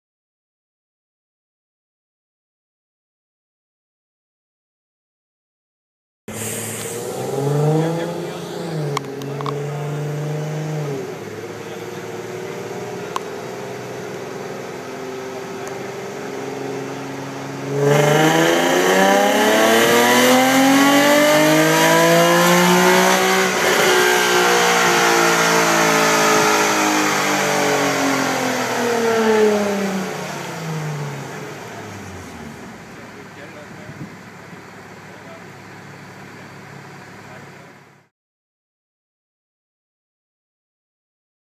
2009 Infiniti FX35 Uprev Tune
A tune at Stillen with the Uprev Osiris